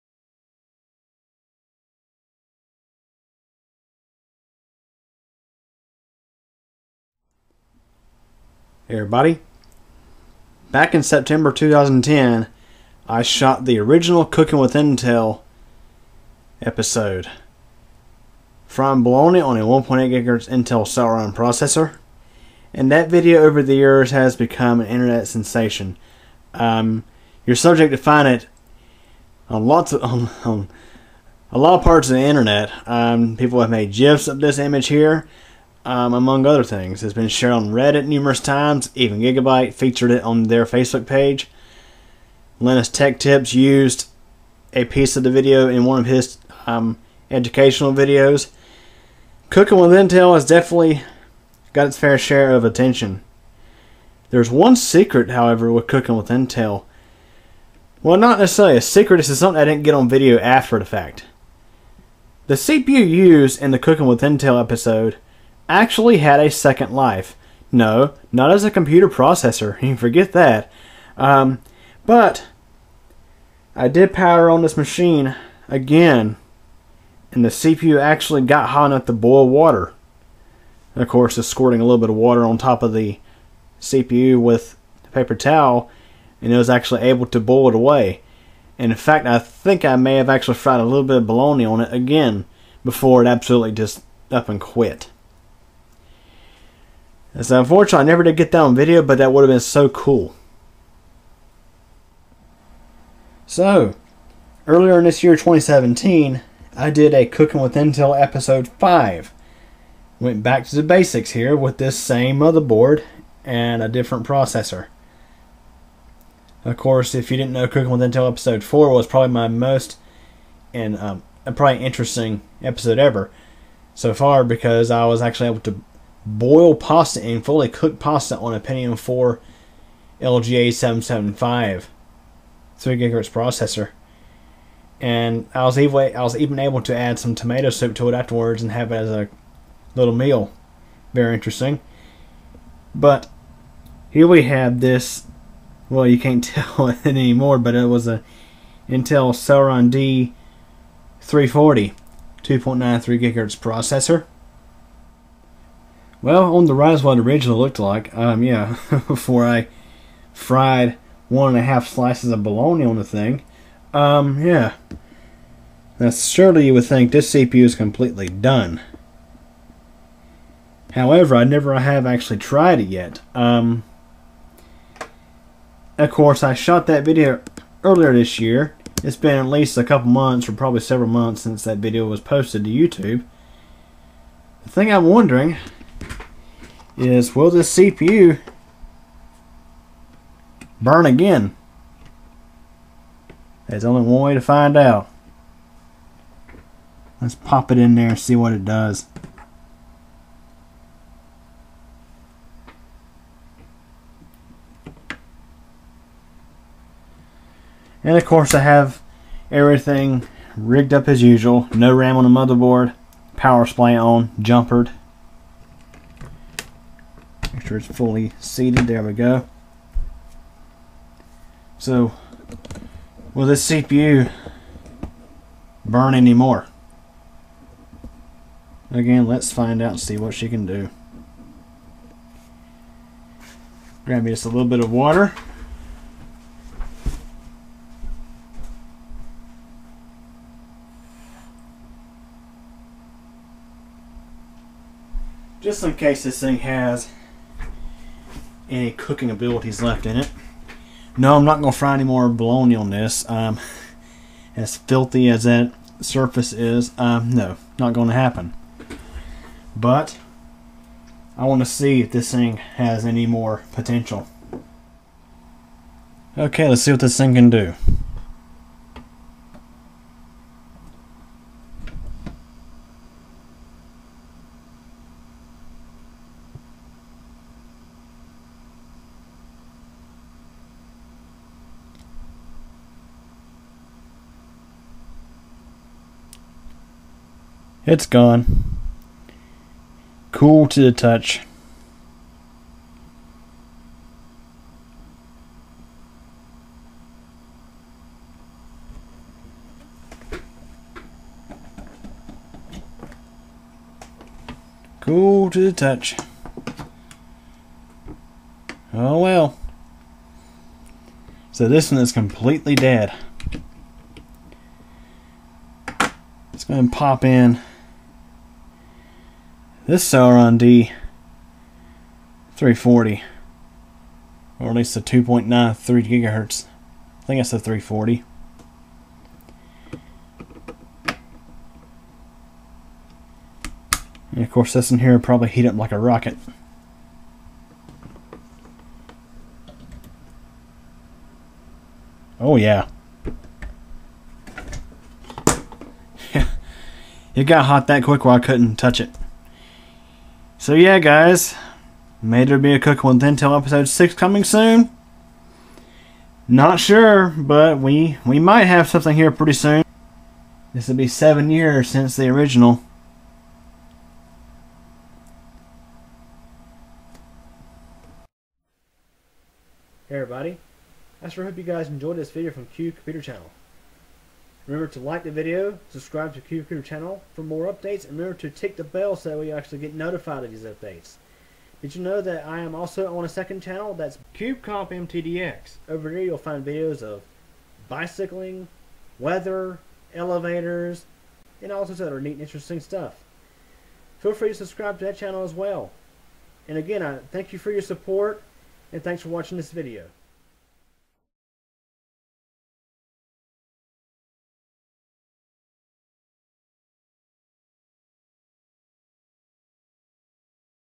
Will the CPU from Cooking with Intel 5 heat up again?
Just for fun, let's see if there's any life left in the burned out CPU from Cooking with Intel 5!.